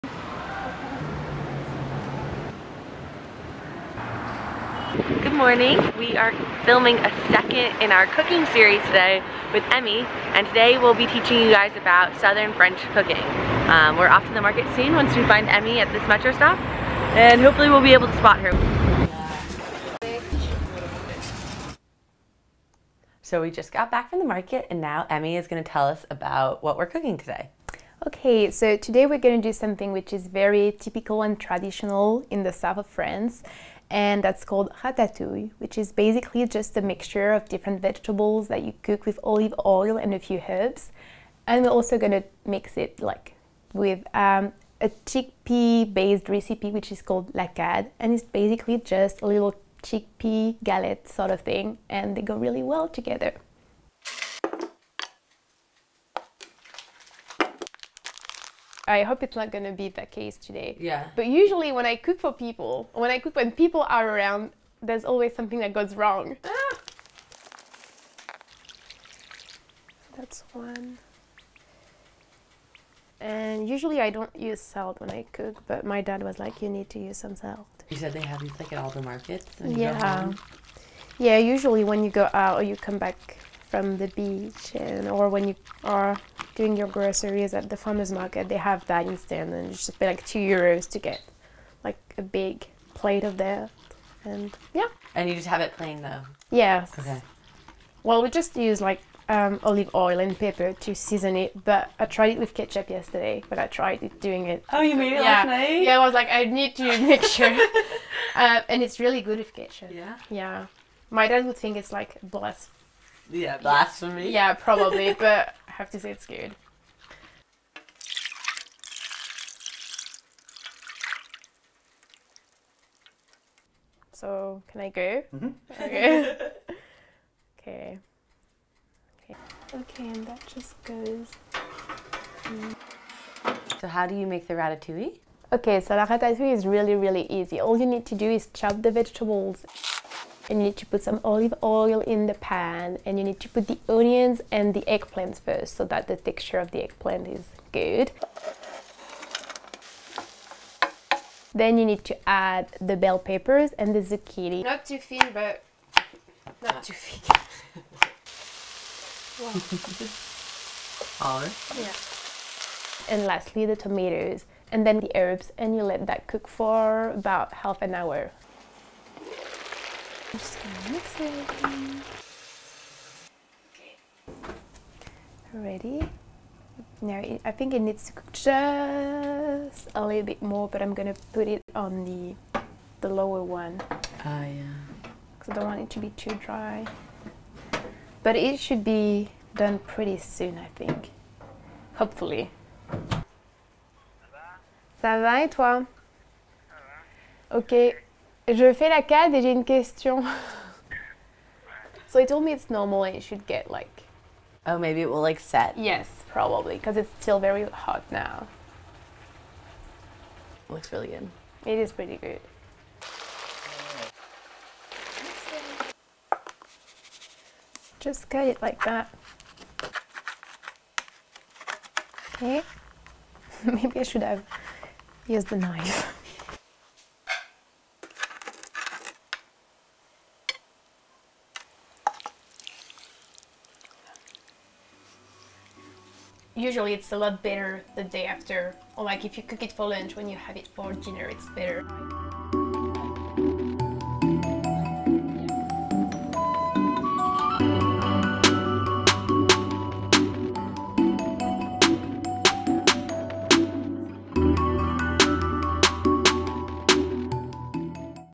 French Vegan Recipe with Antastesia (Emy) in Paris
A French graduate student living in Paris, Emy is one of the most generous and open people Lou and I have ever met! We were thrilled when we found out she had time to hang with us while we were in Paris in September and she even showed us how to make some of her favorite meals inspired by her childhood growing up in Southern France. You can also buy her cooking ebook

Recipes:
Ratatouille
4 tomatoes
1 onion
3 zucchini
1 eggplant
1 red bell pepper
1 yellow bell pepper garlic
fresh herbs &1 tbs olive oil
Add some olive oil the pan, the sliced onion and garlic and let cook until they're golden. Add in the bell pepper and let cook for a little more. Then add the zucchini and eggplant and let cook for 10 minutes more. Keep it on medium heat all the time. Next add the tomatoes! Cook for 40mns/1h on medium/low heat, and then serve.

the cade
25g chickpea flour
1 tbs of olive oil
1/2 liter of water

Mix all ingredients and then cook for 40/45 minutes at 250C
Video Camera: Canon 5D3 with Canon f4 24-105 lens and Leica 35mm Summicron F2 (1970)
Audio: Sennheiser MKE 600 and for the interviews Sennheiser G3 lavalier microphones